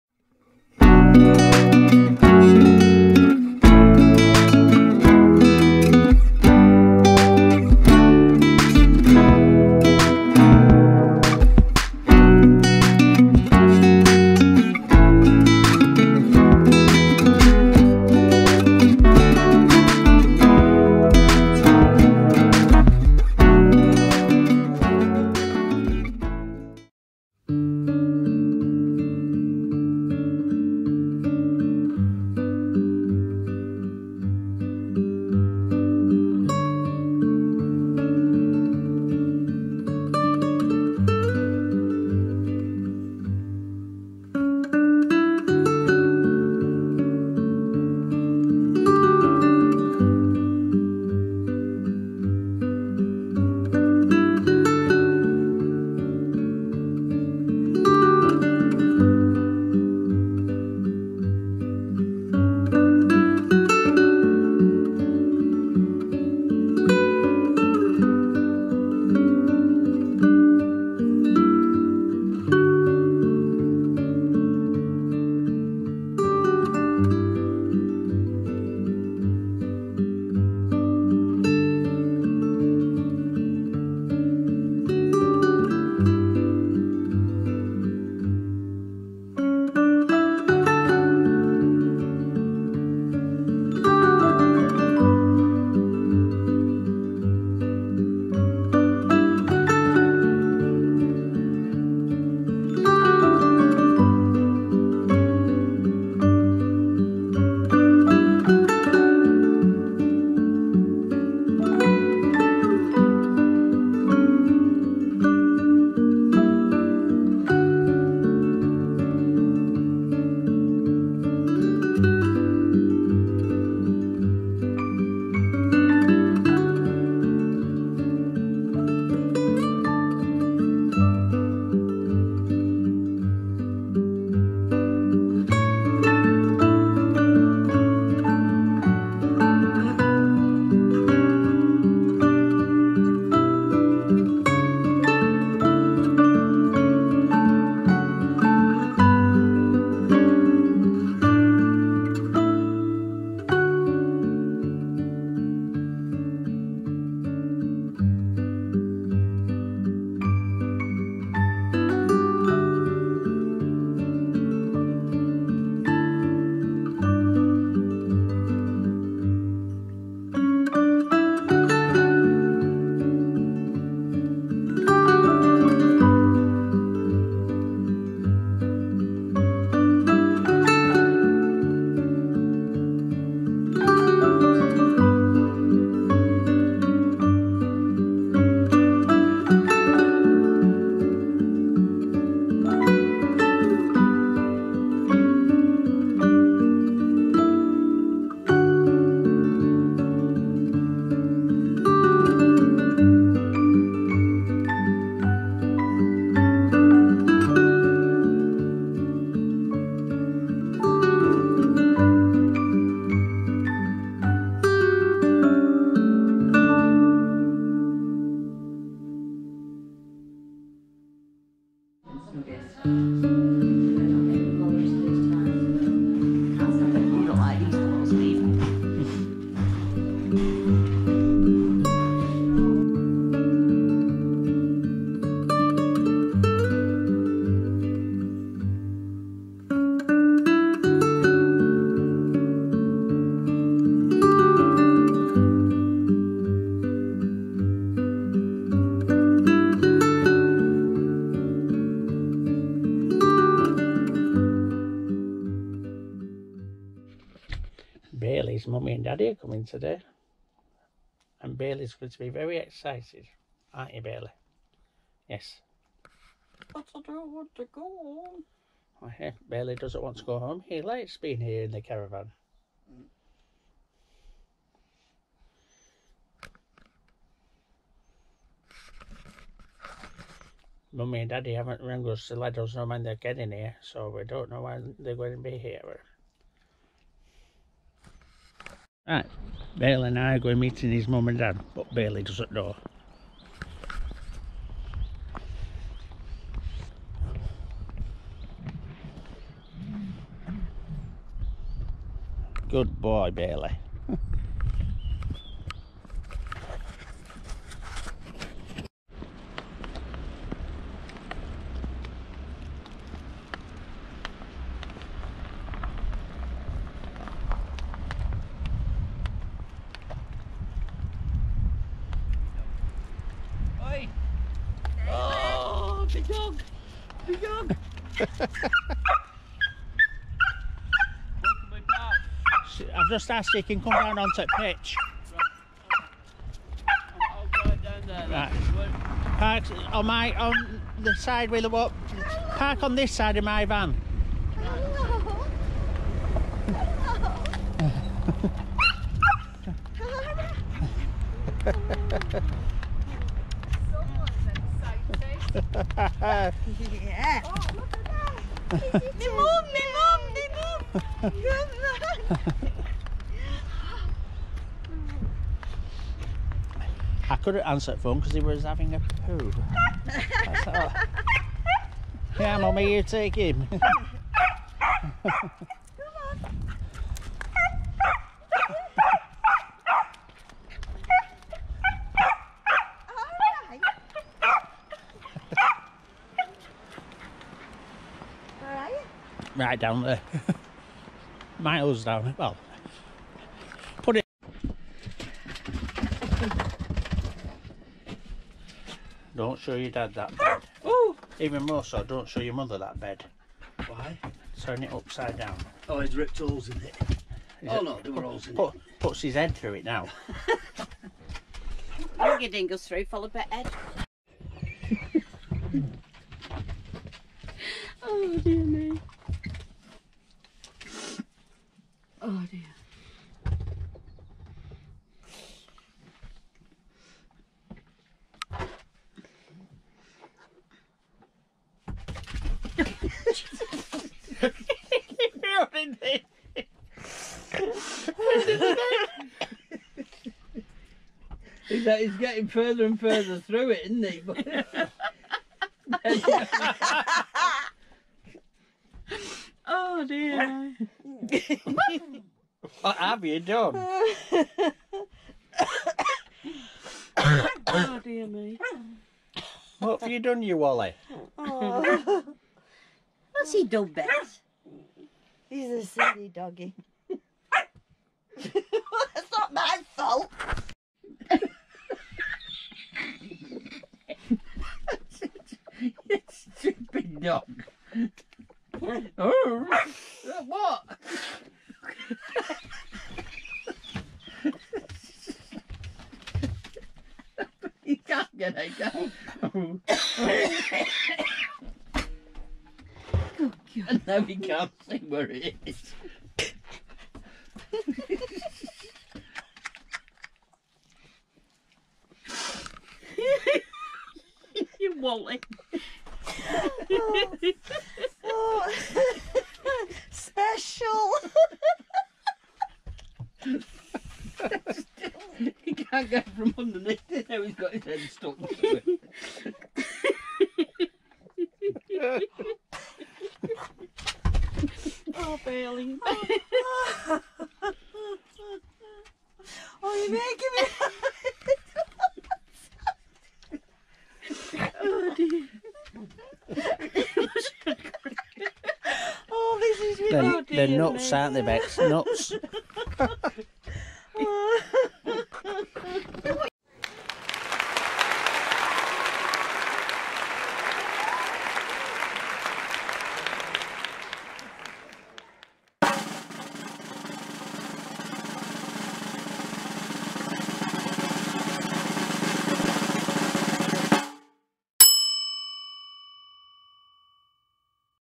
Baileys First Caravan Weekend
In this video we look at Bex Friend Bailey the Dachshund's first weekend in the caravan